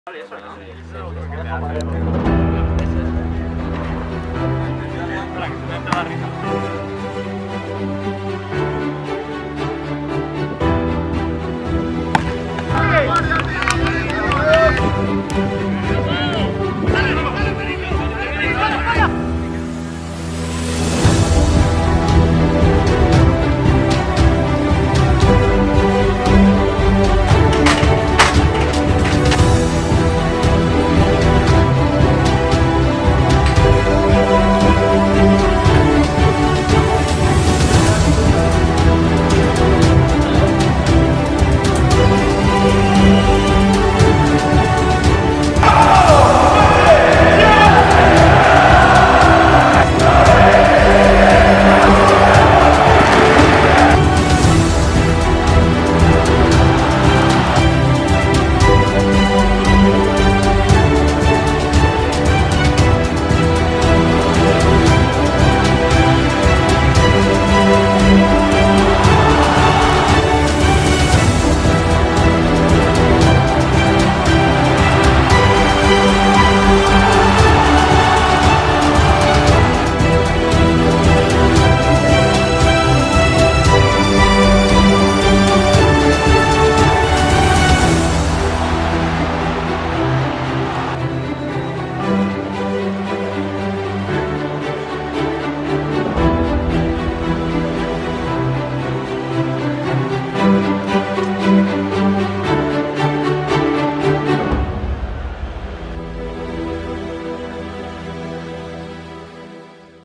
MatchReview | Tanquem una setmana a Astúries | J2 Real Sporting vs FC Andorra
Després d'una setmana a territori asturià, l'equip tricolor tanca la segona jornada de LaLigaSmartbank al camp del Real Sporting.